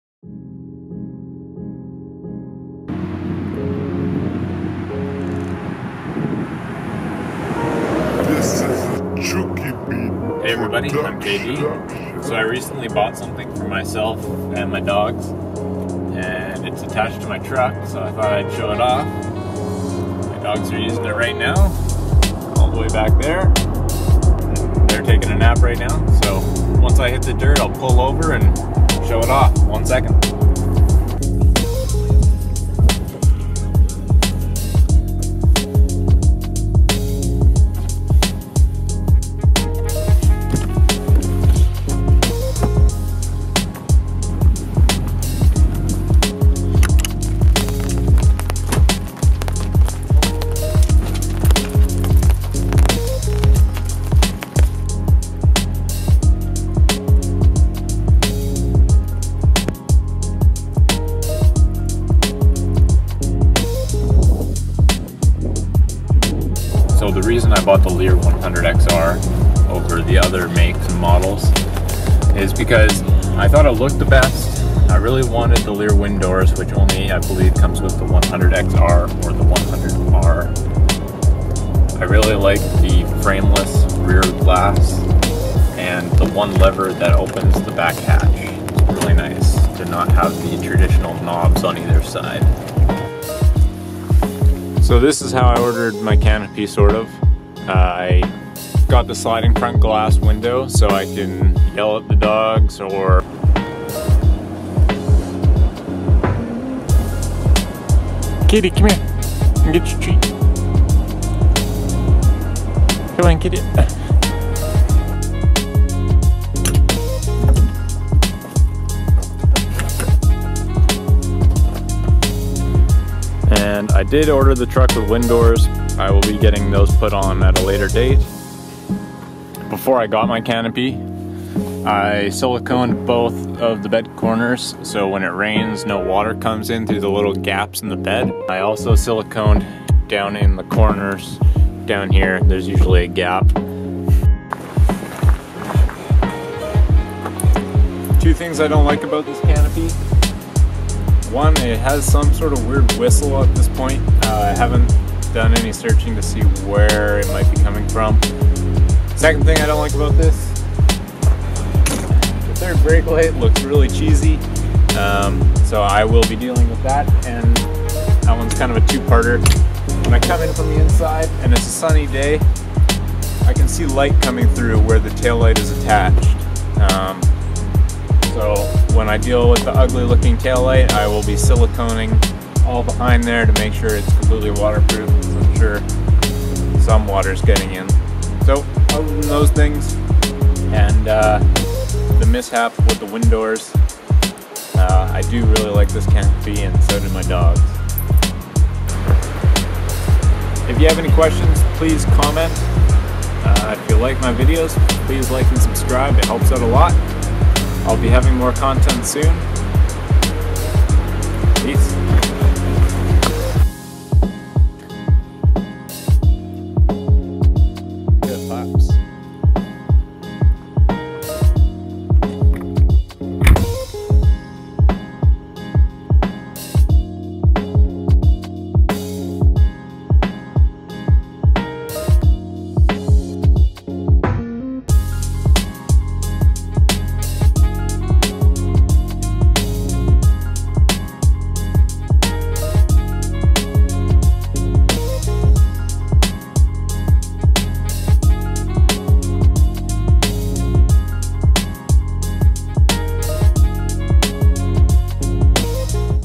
a Mod for my Dogs! Leer 100 XR | 2017 Tacoma TRD OffRoad Build
Just made a quick video going over my canopy. Can't wait to build around this platform.